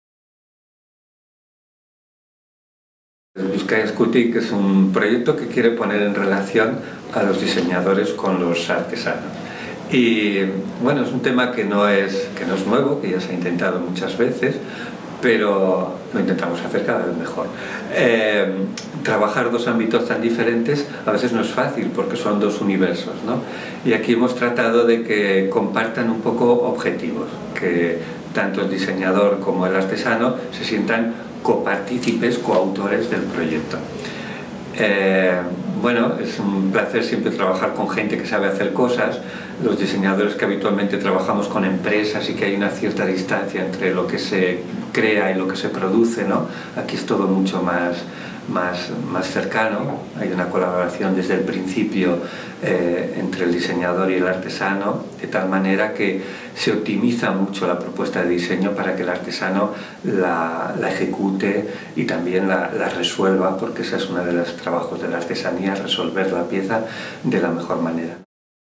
Martin Azua
Proyecto Bizkaia eskutik. Diseñadores y artesanos trabajando conjuntamente